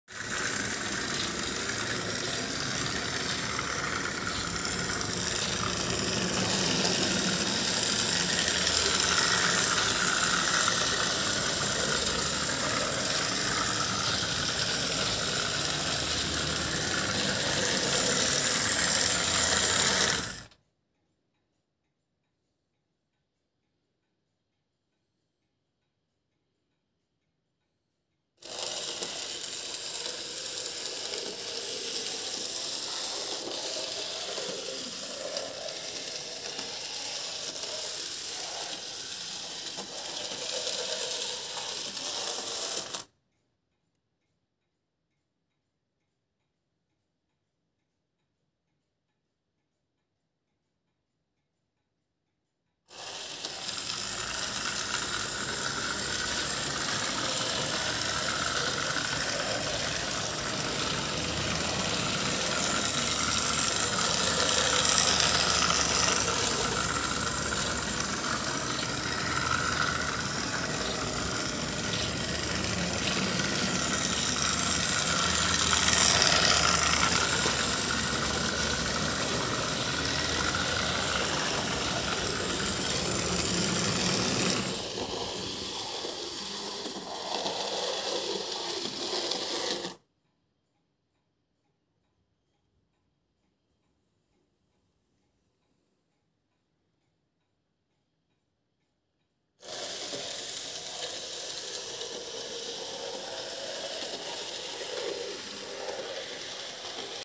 Egger Bahn narrow gauge
Dem Faszinosum der Feld- und Schmalspurbahnen im Maßstab 1:87 konnte man als H0-Modellbahner erstmals in den frühen 60er Jahren erliegen, als die in München ansässige Firma Egger ihre Produkte auf den Markt brachte. Bis heute hat sich nichts an der Ausstrahlung dieser Fahrzeuge und der davon ausgehenden Faszination verändert. In Kombination mit dem EHEIM-Trolleybus entsteht auch auf dieser winzigen Anlage ein Flair, dem sich der Betrachter nur schwer entziehen kann. Viel Freude beim Anschauen!